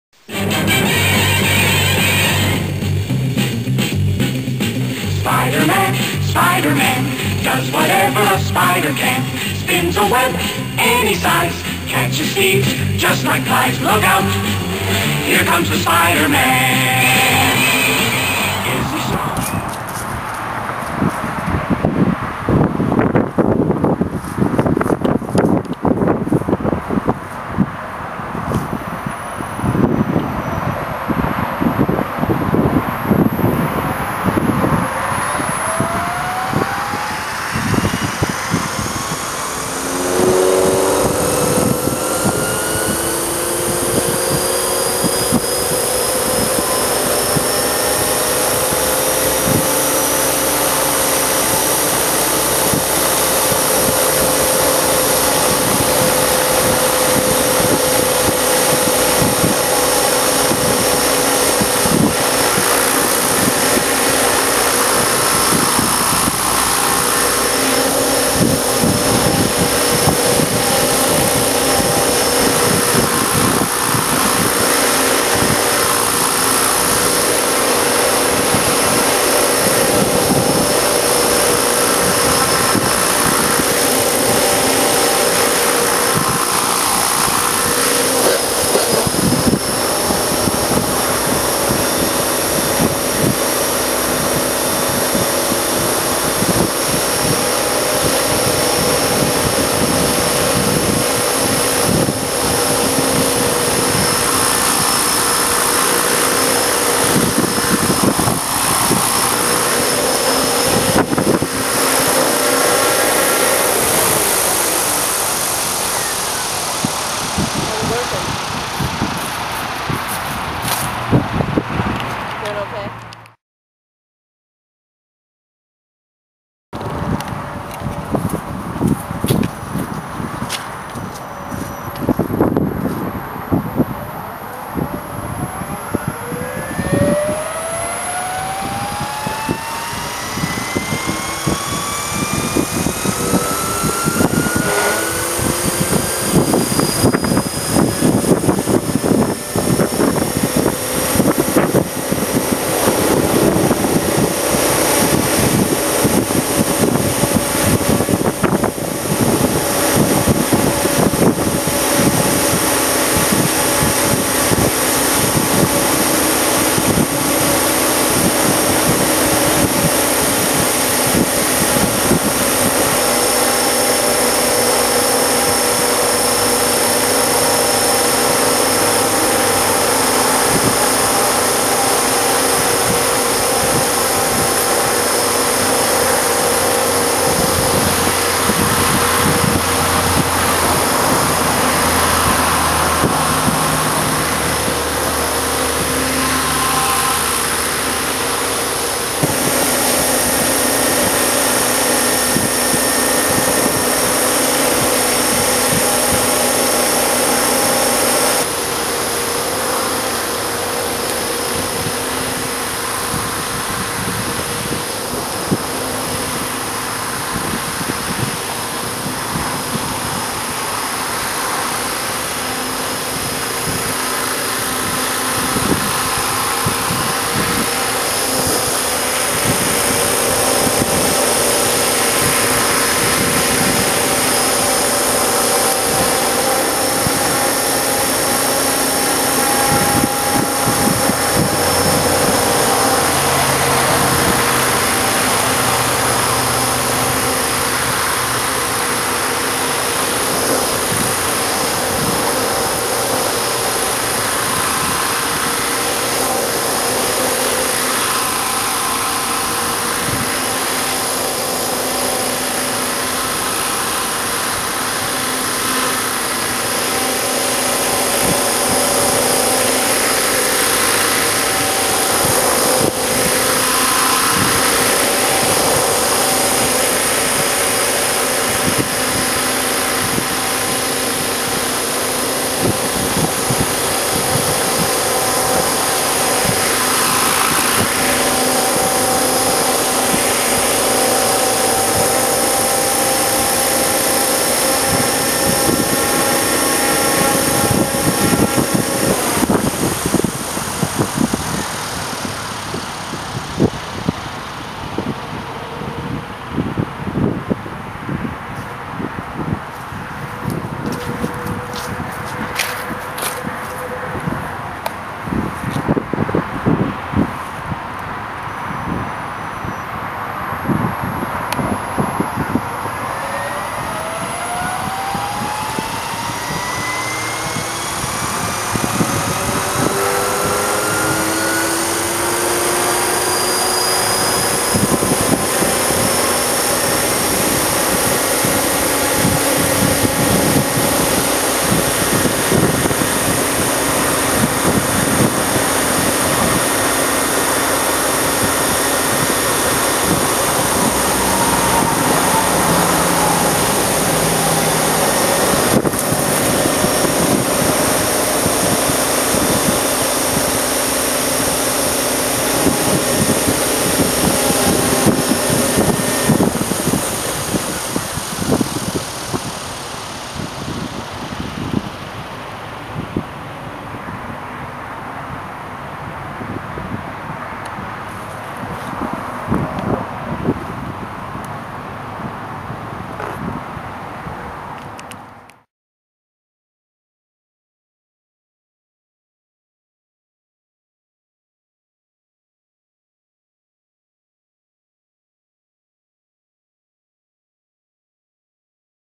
Spiderman Night Flight
Well I finally got my new Spektrum DX7 in the Jet ranger body. Wanted to get a flight in. But the winds today have been over 31mph. Waited till almost dark out, and it never let up. So I grabbed my video camera and flew it in the dark. The landing light can be remotely turned off/on as you can see from the film. I still need to dial it in some more. But as windy as it was it was a nightmare just to keep in in a 10 foot area !